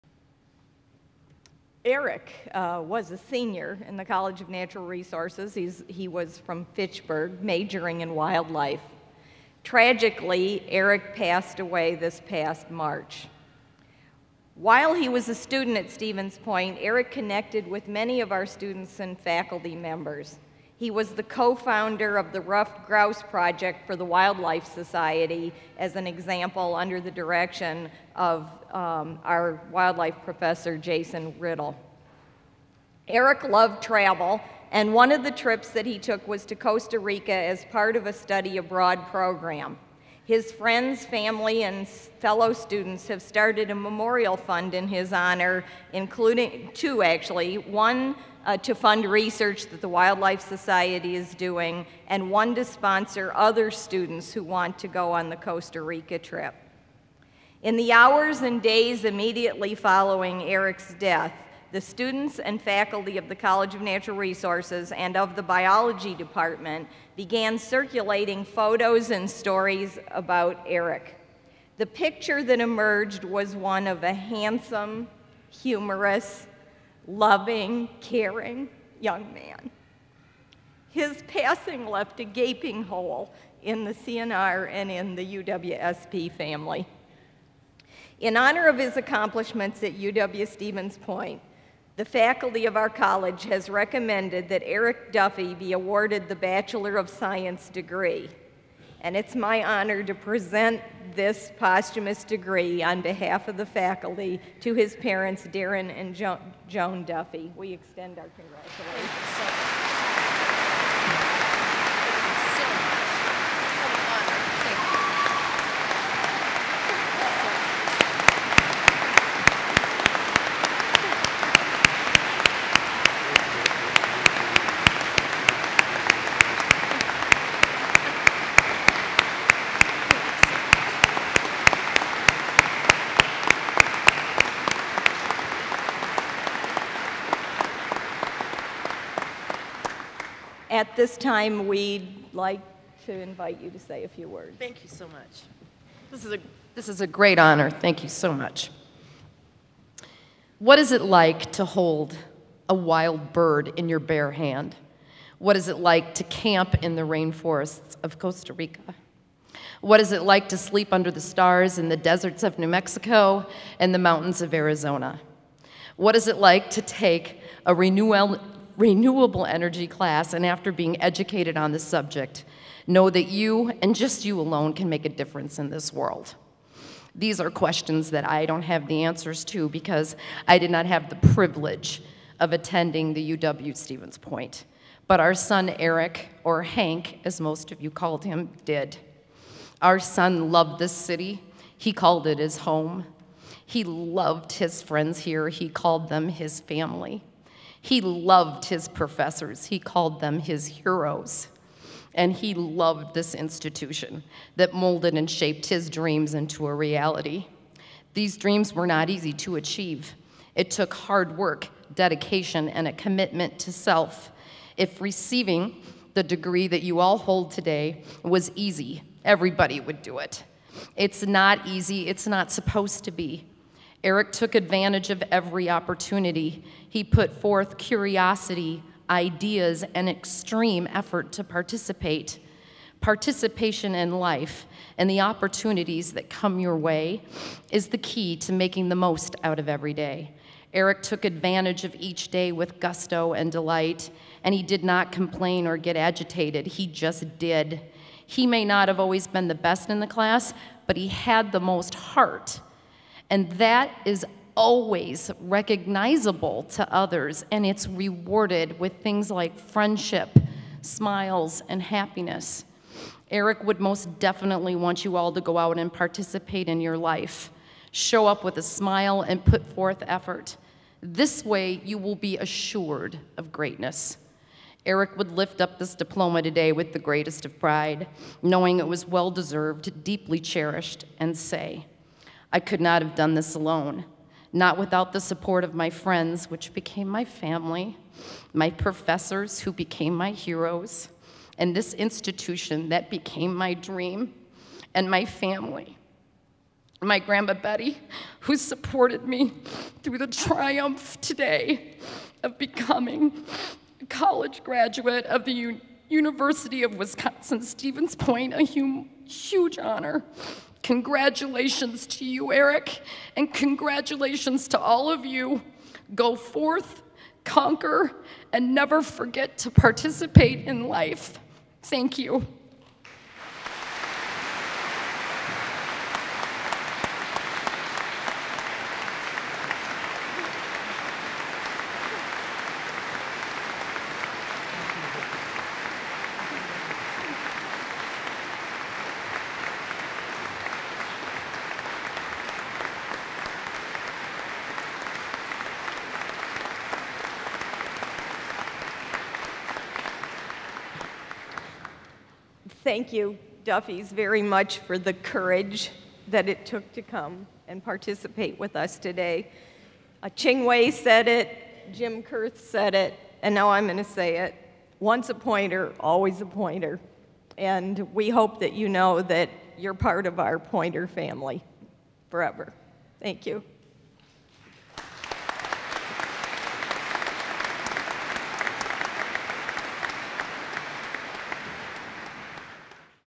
Eric Duffey, Posthumous Bachelors of Science degree
Eric Duffey, a senior majoring in Wildlife Management is awarded a posthumous Bachelors of Science degree.  Christine Thomas, Dean of the College of Natural Resources and Eric's parents, Daren and Joan Duffey, share a glimpse of Eric's life and love of the natural world.